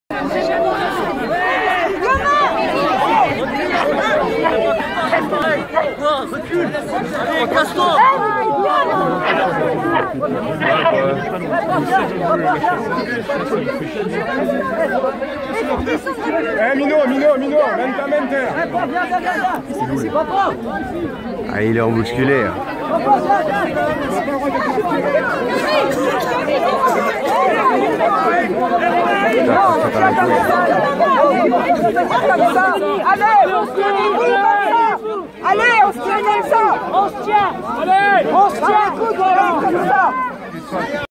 Manifestation contre la Chasse à Courre 31/3/18
150 AVA en forêt de Compiègne, pour manifester contre la Chasse à Courre. La police s'interpose, nous encercle, hélicoptère tourne dans le ciel, 9 camions de CRS et 5 patrouilles de police, rien que pour nous, pourquoi? pour que les chasseurs puissent chasser tranquilles?